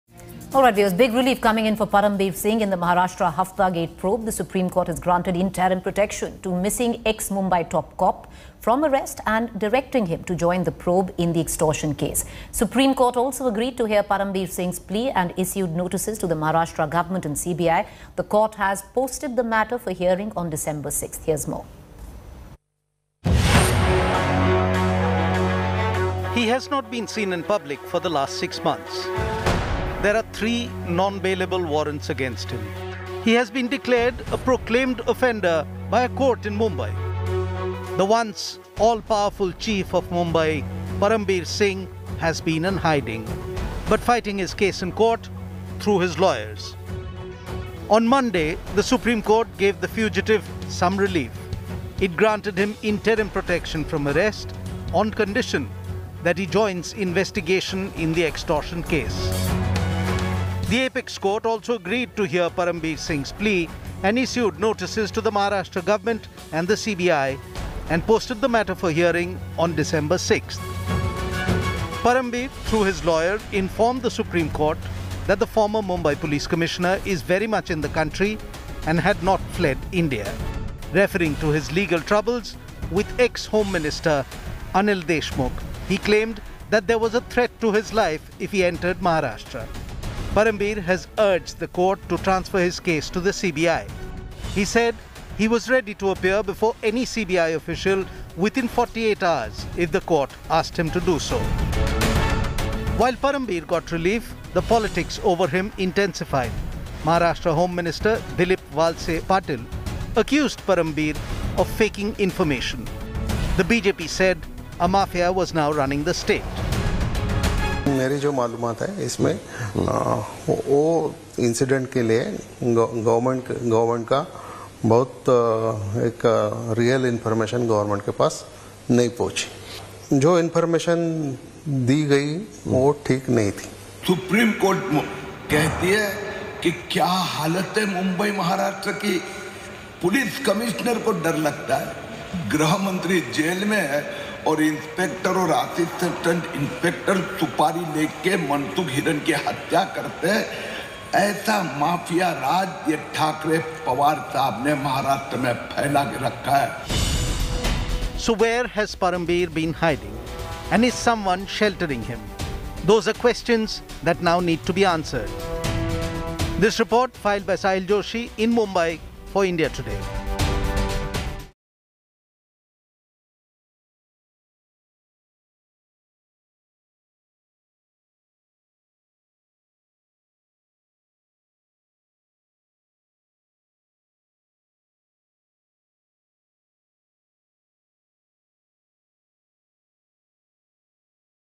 SC Grants Param Bir Singh Relief From Arrest | Maha Hafta Gate Twist
The Supreme Court has granted former Mumbai top cop Param Bir Singh protection from arrest in an alleged extortion case. While granting the same, the court directed Param Bir Singh to join the investigation.